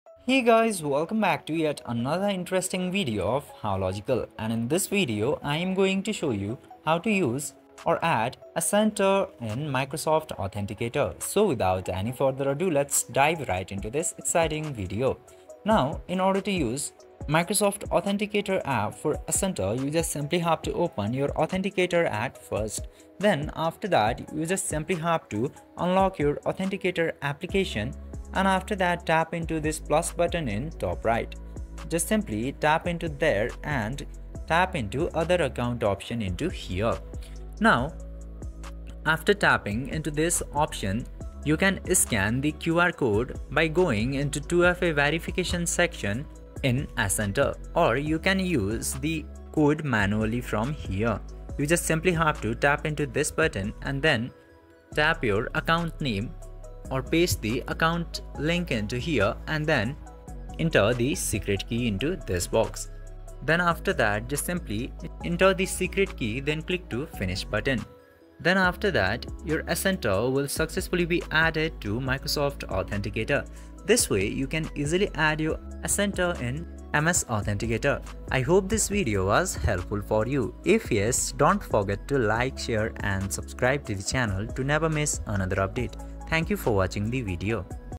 How to Add Accenture to Microsoft Authenticator (2024) | Microsoft Authenticator Tutorial
This tutorial will guide you through the process, ensuring your work profile is protected with two-factor authentication. Add your Accenture account to Microsoft Authenticator for enhanced security.